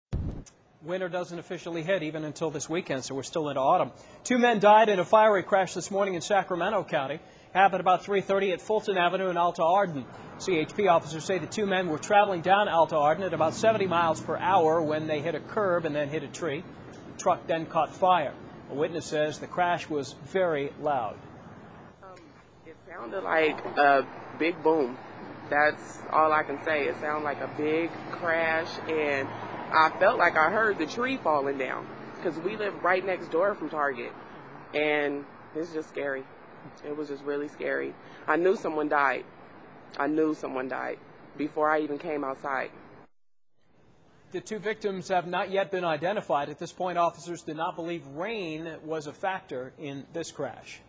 2 Killed In Sacramento Pickup Crash
Two men died early Monday in a fiery pickup truck crash in Sacramento, police said.